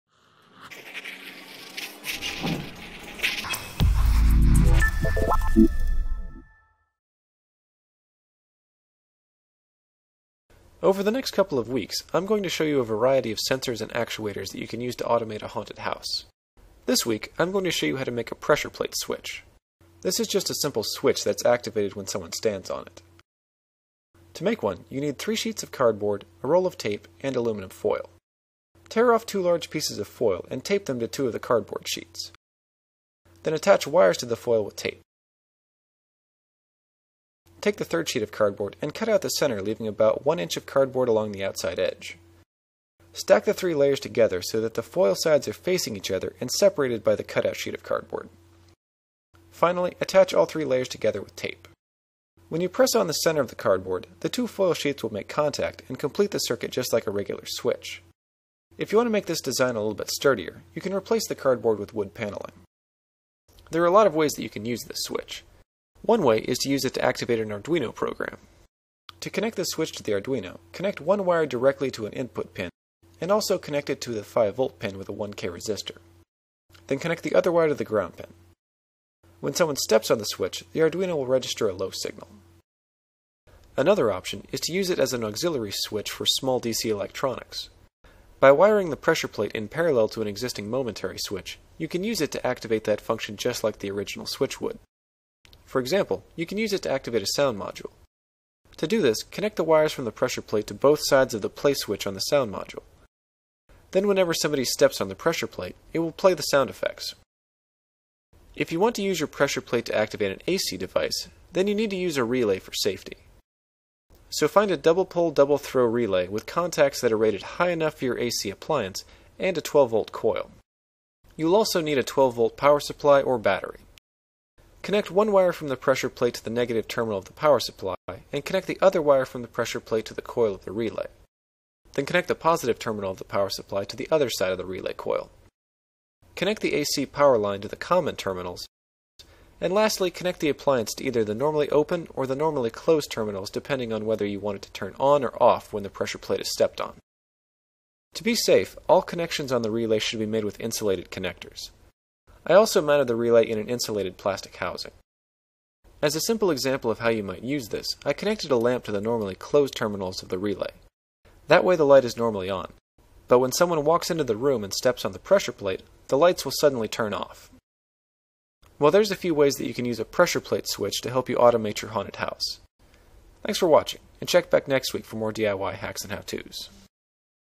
DIY Hacks & How To's: Pressure Plate Switch
One of the best parts of a haunted house is the automated special effects. To make sure that the effects are well timed, you need sensors to detect where your guests are in the haunted house.

Over the next few weeks I am going to show you a variety of simple sensors that you can use to automate your Halloween setup.